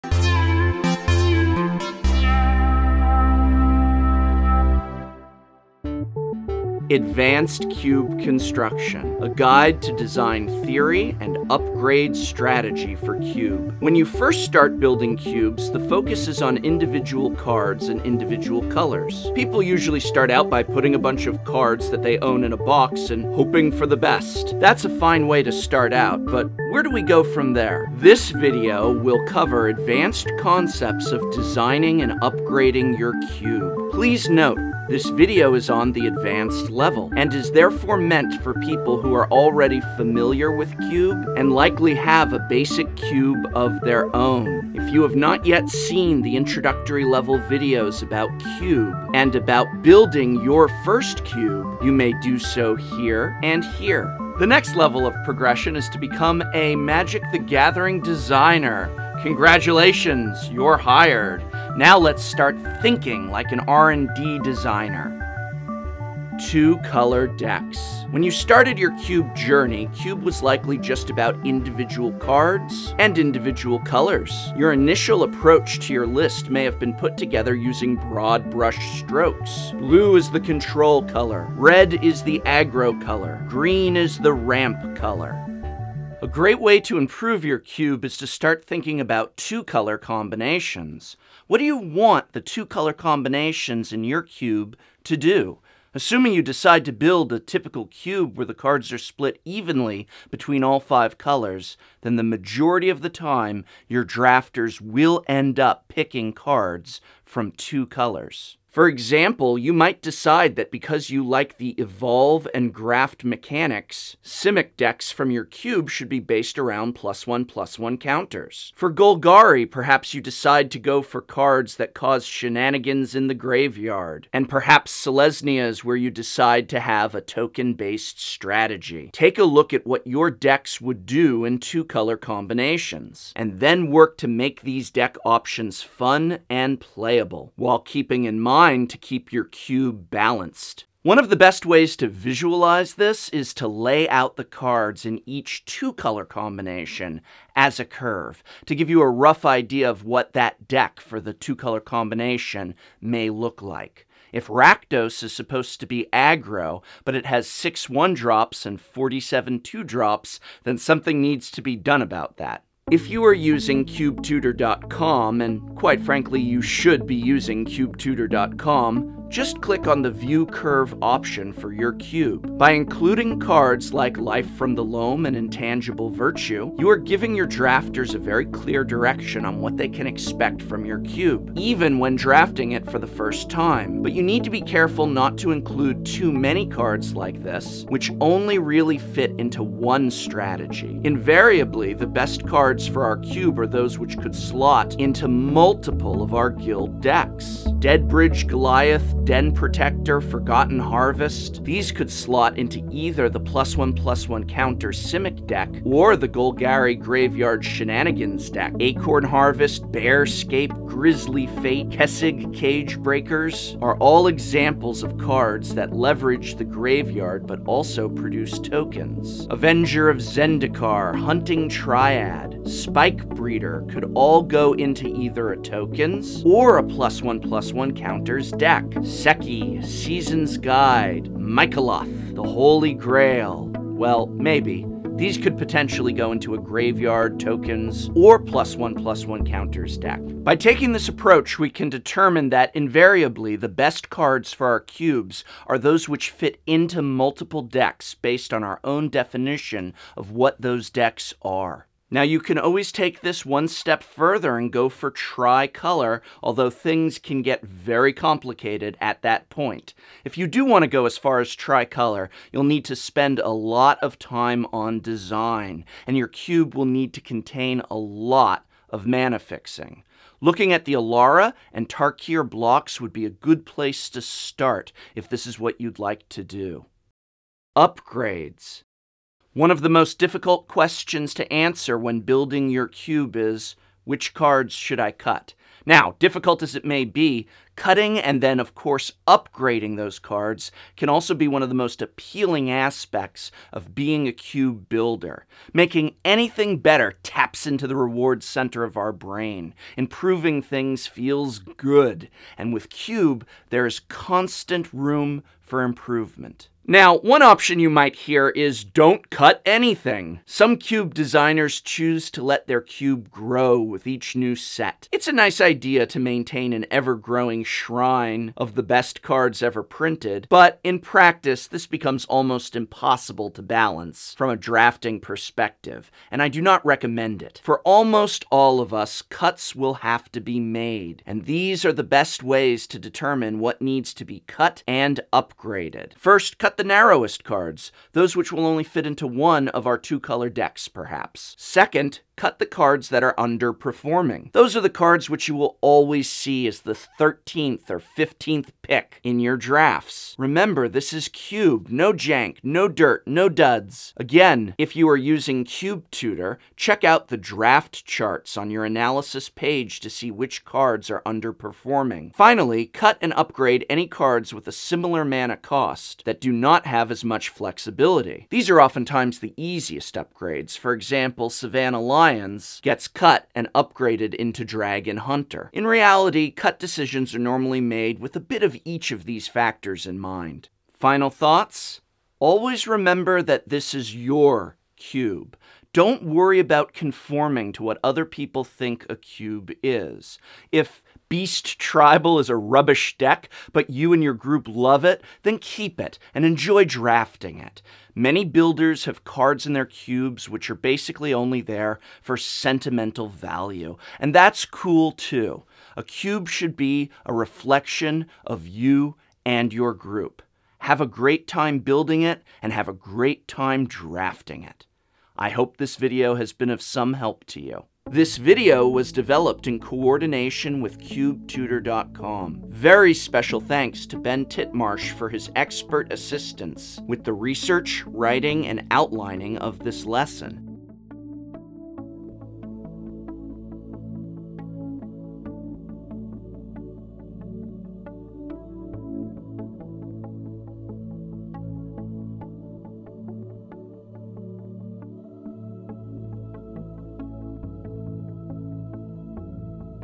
MTG - Advanced Cube Construction 301 - Design And Upgrade Your Cube For Magic: The Gathering
Advanced Cube Construction, a guide to Design theory and Upgrades strategy for Cube.  When you first start building cubes, the focus is on individual cards, and individual colours.  People usually start out by putting a bunch of cards that they own in a box and hoping for the best (albeit ensuring that there are an equal number of cards in each colour).  Or they'll by constructing a "Best of Magic The Gathering" style list with all of the best cards ever printed.  That's a fine way to start out, but where do we go from there?

This video will advanced concepts of designing and upgrading your cube.  Please note: this video is on the advanced level, and meant for people who already familiar with cube, and likely have a basic cube of their own.

When you started your cube journey, cube was likely just about individual cards and individual colors.  Your initial approach to your list may have been put together using broad brush strokes - “Blue is the Control Color”, “Red is the Aggro Color”, “Green is Ramp” - Perhaps you could show some stereotypical cards at this point, Counterspell, Goblin Guide, Llanowar Elves etc.

A great way to improve your cube is to start thinking about two color combinations.  What do you want the two color combinations to do?  Assuming you decide to build a typical cube where the cards are split evenly between all five colors, then the majority of the time your drafters will end up picking cards from two colors.